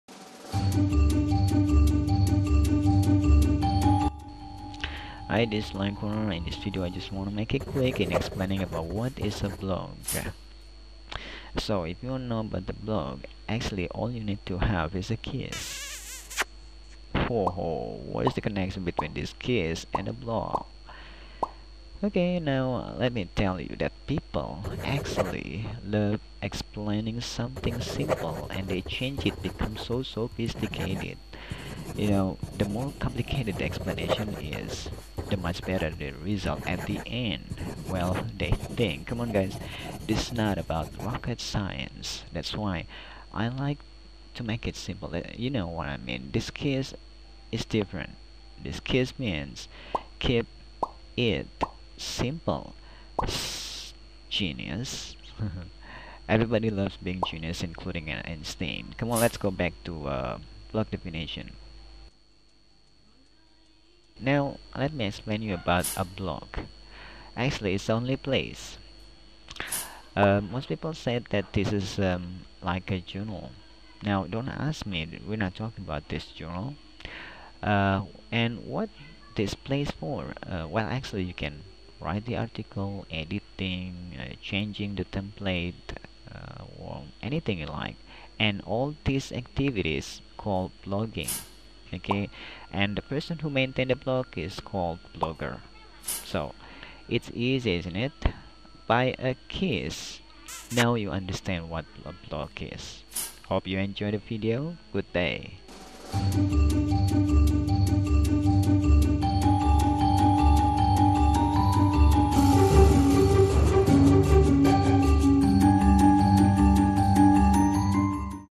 what is a blog ?
easy explanation about what is a blog . All you need to know is how to get a kiss !! confused ? just watch the video of  " what is a blog? " it's a simple answer about blog, blogging, and blogger.